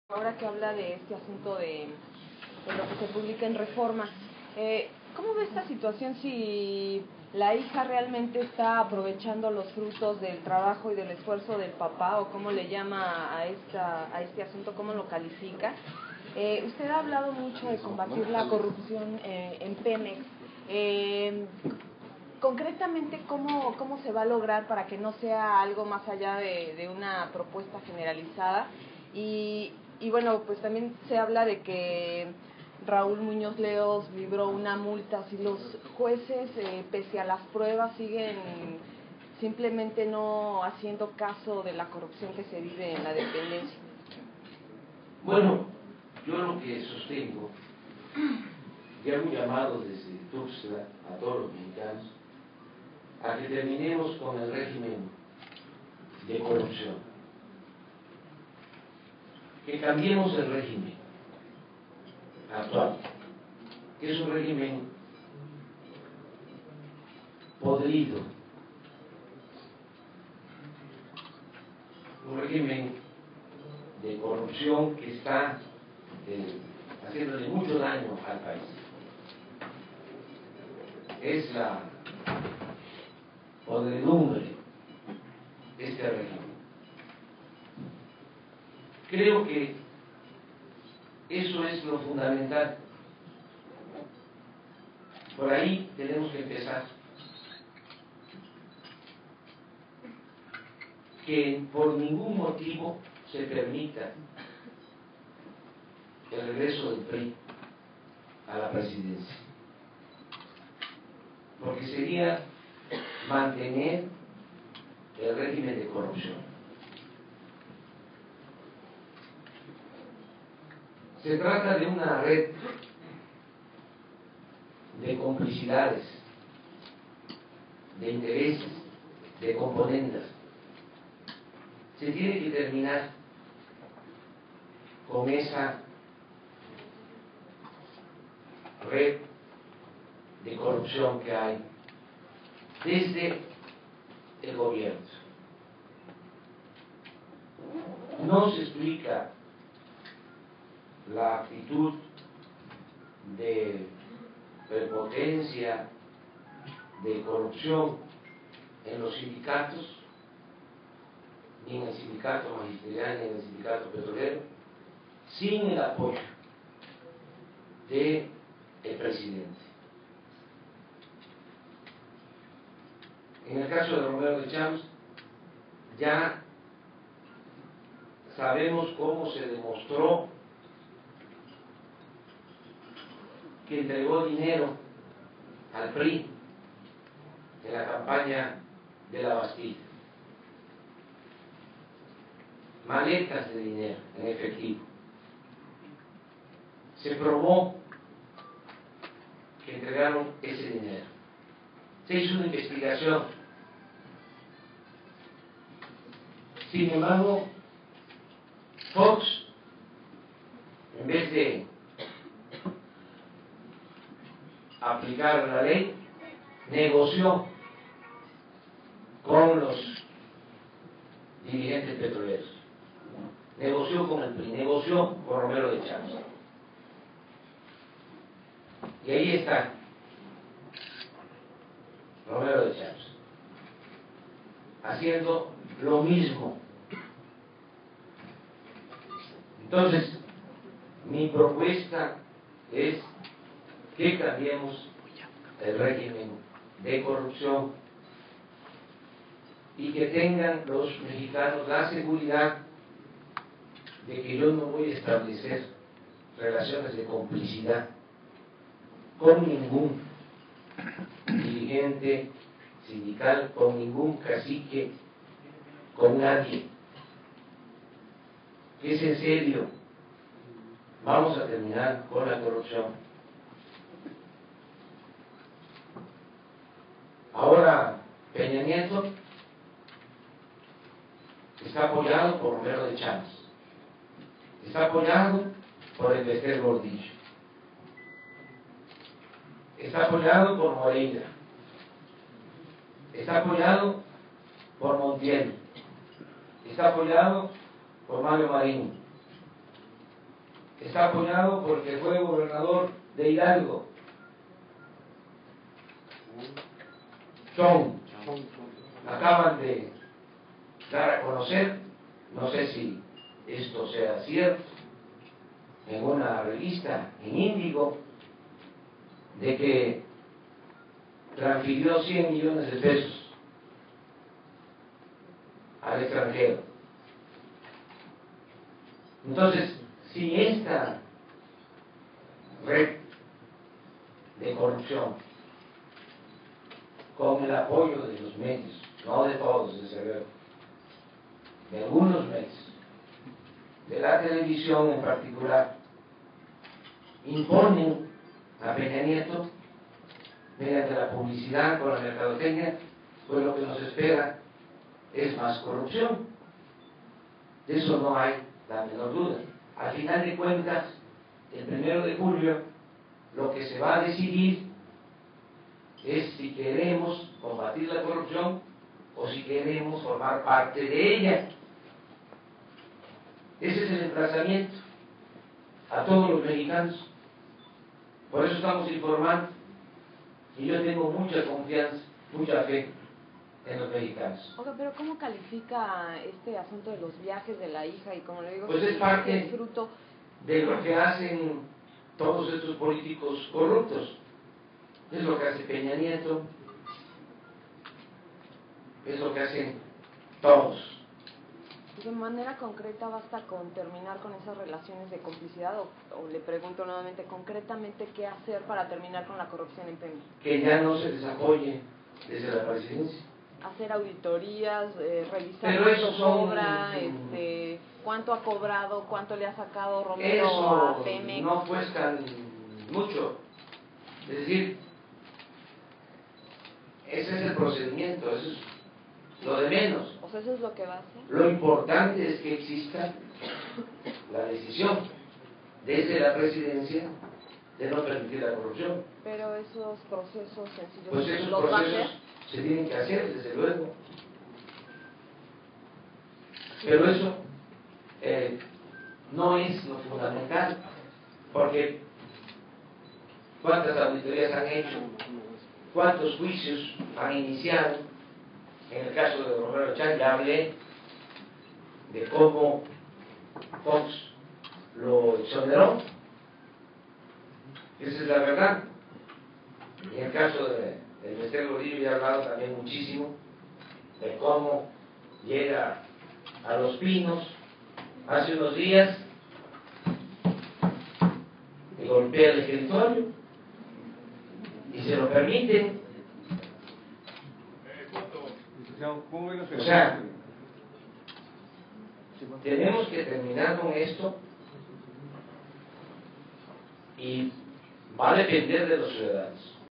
AMLO líderes como Romero Deschamps dañan País
El candidato de izquierda Andrés Manuel López Obrador afirmó que líderes sindicales como Carlos Romero Deschamps dañan al País y que, de ganar la Presidencia, efectuará auditorías para conocer cómo se ha enriquecido y acabará con sus privilegios. Al iniciar su conferencia matutina, el tabasqueño mencionó el caso de la hija del dirigente petrolero, Paulina Romero, quien presume viajes por el mundo y come en restaurantes exclusivos, cuando su padre gana como obrero 24 mil 633 pesos.

Afirmó que este es un ejemplo de cómo viven los líderes sindicales corruptos y ex Gobernadores priistas que rodean a su adversario Enrique Peña Nieto, como Romero Deschamps, Elba Esther Gordillo, Arturo Montiel, Mario Marín y Miguel Ángel Osorio Chong. "No pueden estar ya estos dirigentes que hacen tanto daño al País, hace falta un cambio, una renovación, así lo quieren los trabajadores, no voy yo a destituir, van a ser los trabajadores, quienes tendrán libertad para elegir, habrá democracia sindical", dijo. "Que tengan los mexicanos la seguridad que no voy a establecer relaciones de complicidad con ningún dirigente sindical, es en serio, vamos a terminar con la corrupción". Advirtió que de llegar a la Presidencia realizará auditorías para conocer cuánto se ha llevado el líder sindical en Pemex. "Esto (de la hija) es parte de lo que hacen los políticos corruptos, es lo que hace Peña Nieto (...) hacer auditorías y ver cuánto ha sacado son procesos que se tienen que hacer", señaló.

Al cuestionarle si hará todo lo posible para sacar a Romero Deschamps, se limitó a decir que impulsará que en el sindicato petrolero haya voto libre para que elijan a su líder porque actualmente, agregó, no se permite el sufragio secreto. REFORMA publica hoy que Paulina Romero Deschamps, la hija de Carlos Romero Deschamps, líder del sindicato petrolero --quien oficialmente gana como obrero 24 mil 633 pesos mensuales, incluyendo prestaciones-- vive en realidad, como la hija de un jeque. Ella presume sus viajes por todo el mundo, en aviones comerciales y privados; paseos en yates, comidas en restaurantes exclusivos y beber vinos Vega Sicilia de más de 10 mil pesos la botella. El diario de viajes está en su página de Facebook, donde aparece como "Pauli Rod", y donde también presume bolsas Hermés de miles de dólares, las que considera sus "bolsas del Superama